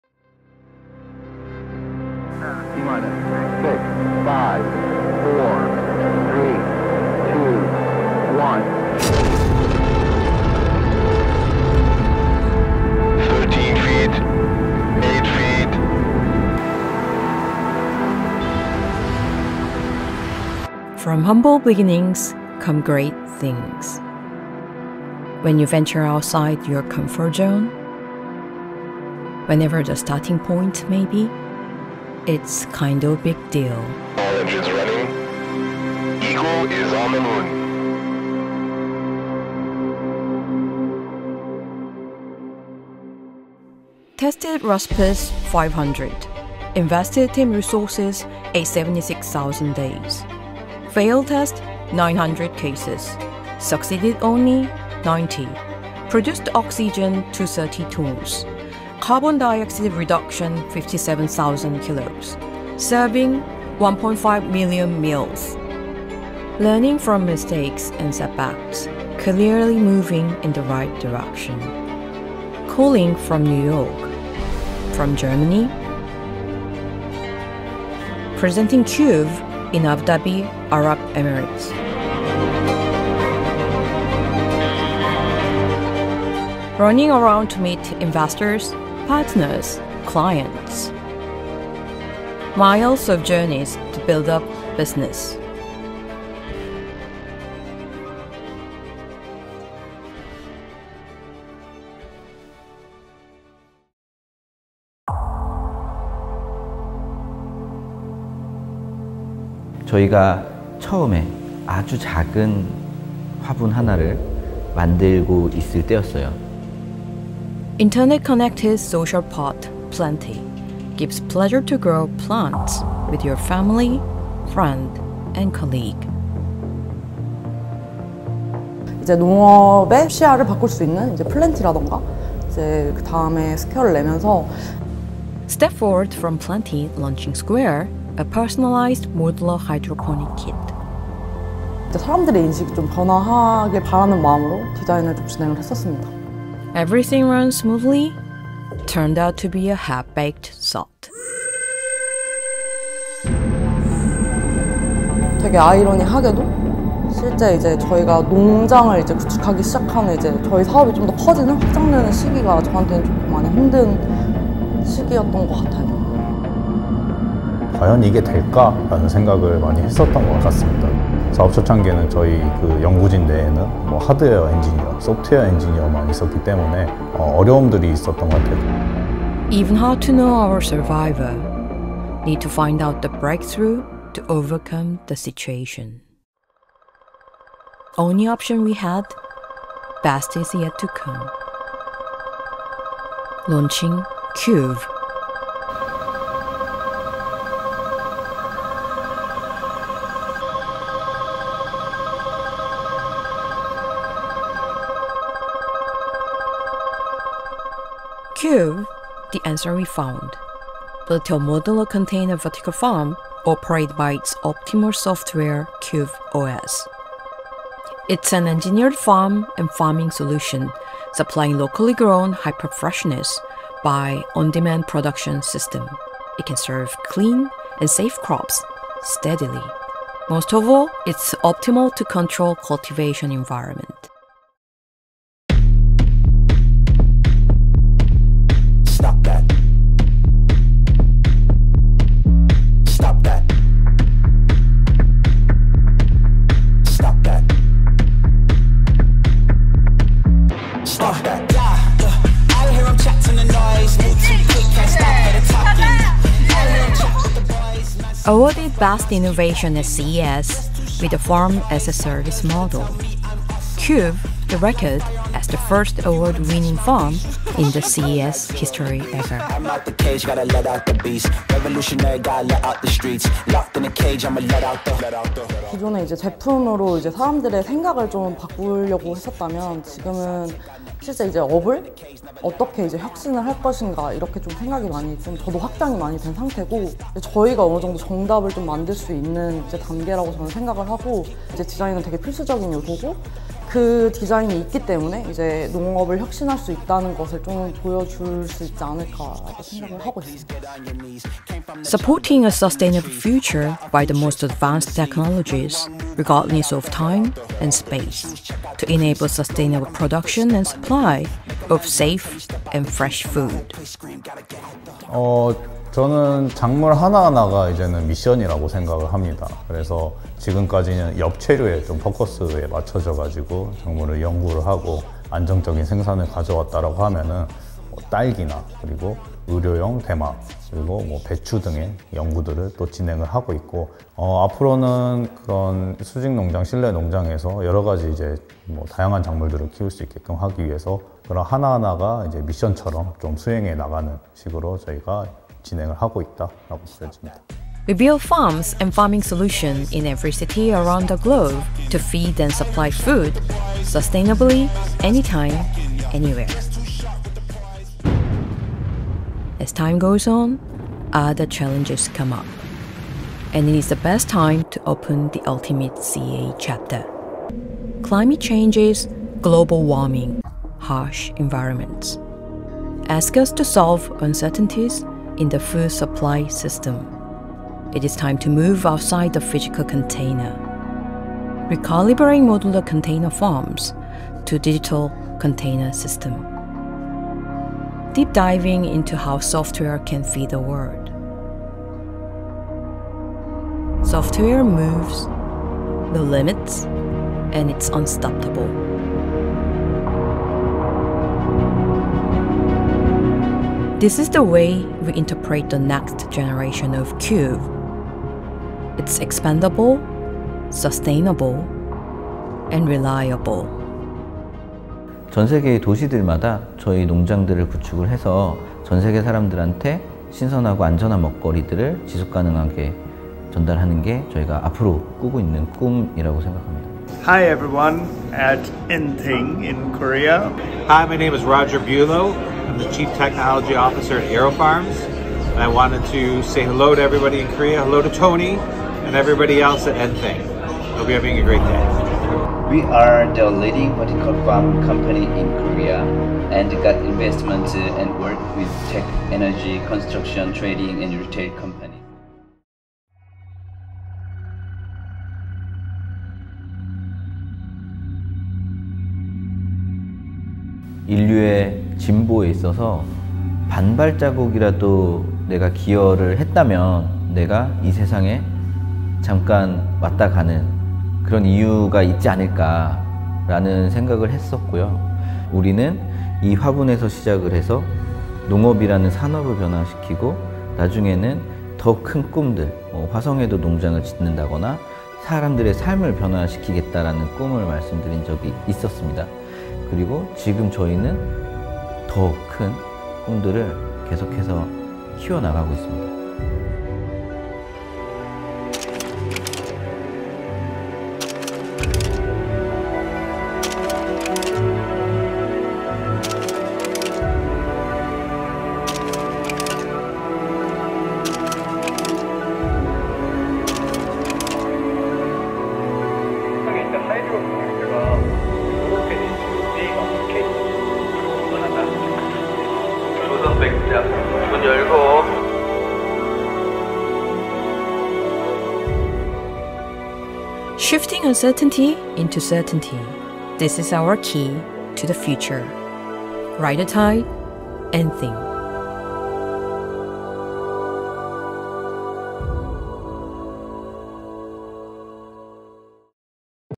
2023 N.THING ORBIT_Ride the Tide _엔씽의 과거, 현재, 그리고 미래
2023 엔씽 오르빗, 작은 화분에서 컨테이너 농장에 이르기까지, 그리고 앞으로의 엔씽은 어떤 모습일까요? 신선하고 안정적인 미래 먹거리에 대한 진지한 고민을 함께 해결해가는 엔씽으로 거듭나겠습니다! N.THING's orbit forever; our orbit since 2014, it's a long but adventurous journey to feed the world by software. More things come up in 2023, and it's our right time to present our next chapter. Ride the Tide!